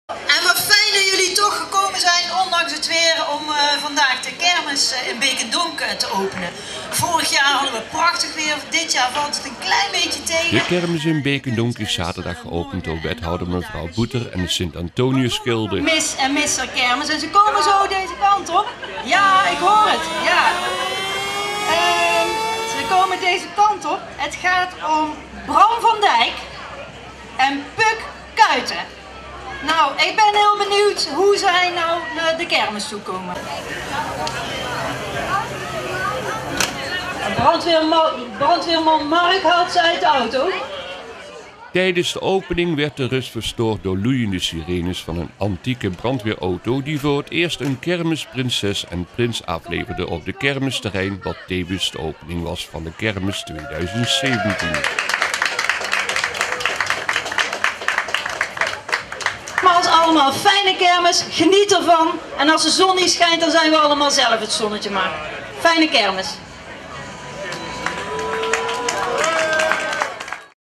2017 09 09 BD Kermis Beek en Donk
Beek & Donk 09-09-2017 Opening Beek & Donk Kermis

De kermis in Beek & Donk is zaterdag geopend door wethouder mevr. Buter en het St Antonius gilde.
Tijdens de opening werd de rust verstoord door loeiende sirenes van een antieke brandweer auto die voor het eerst een kermis Princes en prins afleverde op het kermis terrein wat tevens de opening was van kermis 2017